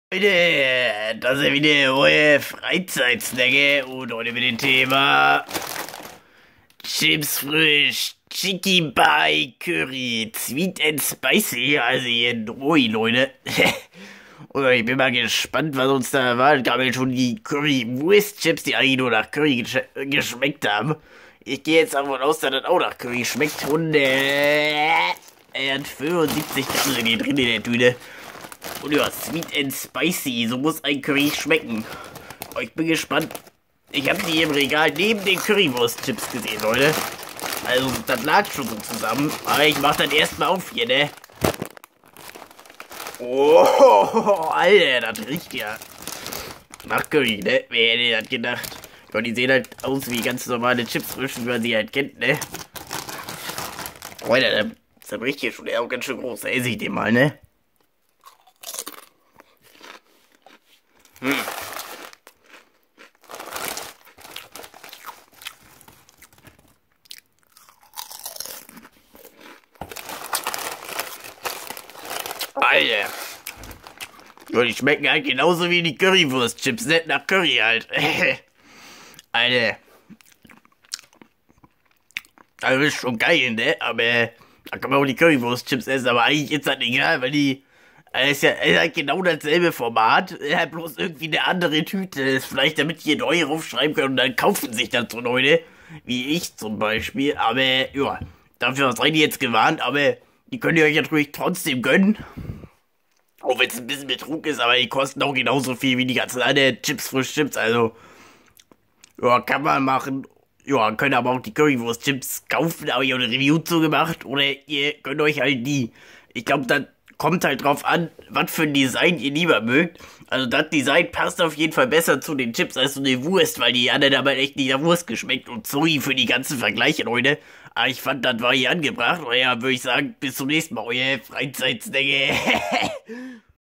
funny-frisch Chipsfrisch Chikibai Curry - Chips Review [Deutsch]
Jo Freunde,

Pikante Chili trifft auf süßliche Mango.
Chipsfrisch Chikibai ist eine fruchtig-scharfe Genusssensation,
die dank milder Chili-Schärfe und fruchtig-süßer Mango für einen
unwiderstehlich, exotischen Geschmack sorgt.

MfG Euer FreizeitSnacker :)